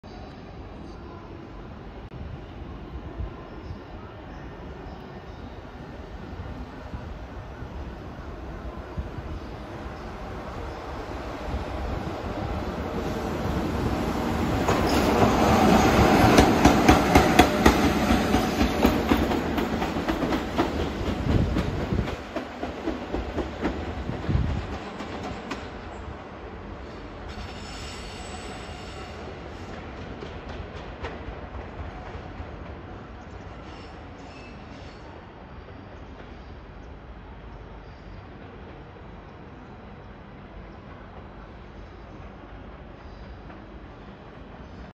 ジョイント音最高！【EF66-100 単機回送】JR貨物EF66形100番台116号機が単機で大宮駅を通過するシーン（4093レ・吹A31・コンテナ輸送）2021.11.7
日曜定期
このジョイント音はハマる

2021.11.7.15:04